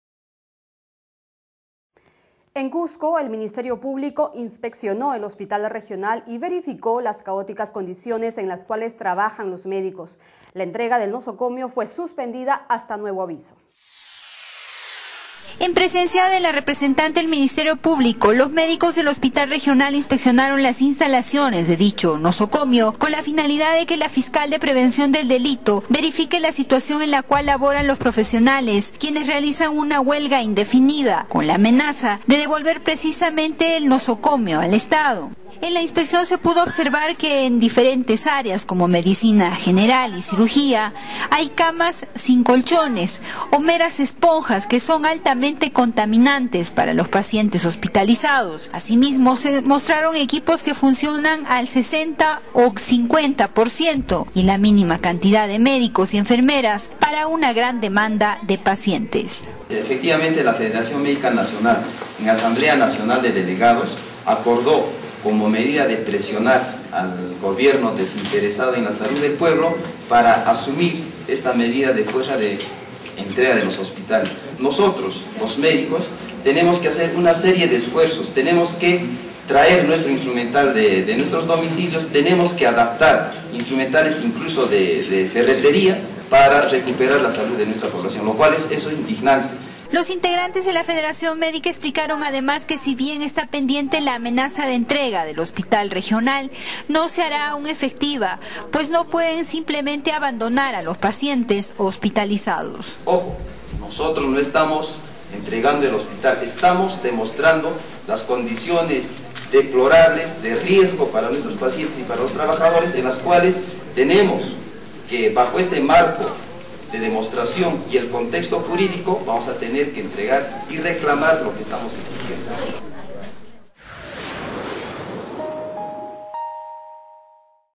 Verifican graves condiciones de trabajo para médicos y pacientes en el Hospital Regional del Cusco
En Cusco, el Ministerio Público inspeccionó el Hospital Regional y verificó las caóticas condiciones en la cuales trabajan los médicos. La entrega del nosocomio fue suspendida hasta nuevo aviso.